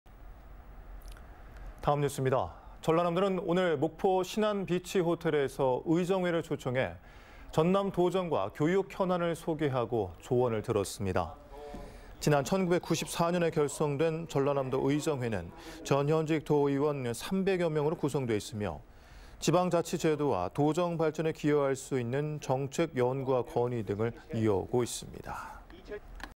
전남도, 의정회 초청 도정 설명회 개최[목포MBC 뉴스데스크]
전라남도는
오늘 목포신안비치호텔에서 의정회 초청해
전남 도정과 교육현안을 소개하고
조언을 들었습니다.

 지난 1994년에 결성된
전라남도 의정회는 전·현직 도의원
3백여 명으로 구성돼있으며
지방자치제도와 도정 발전에 기여할 수 있는
정책 연구와 건의 등을 이어오고 있습니다.